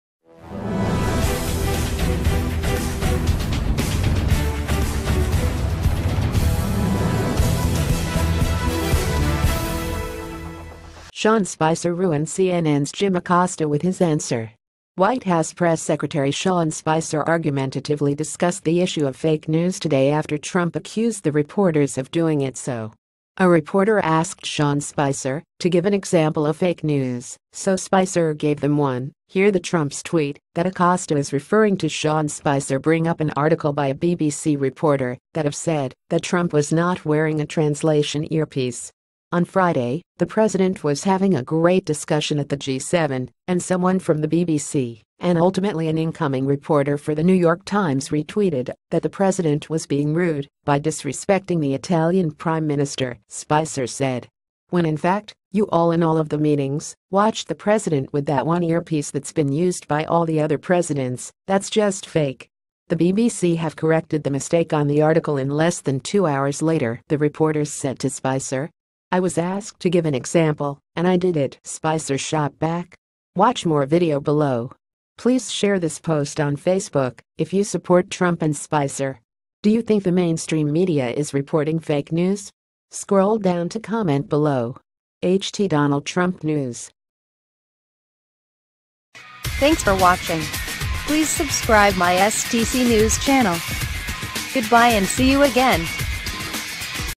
Sean Spicer Ruined CNN’s Jim Acosta With His Answer - STC NEWS
Hot news - Sean Spicer Ruined CNN’s Jim Acosta With His Answer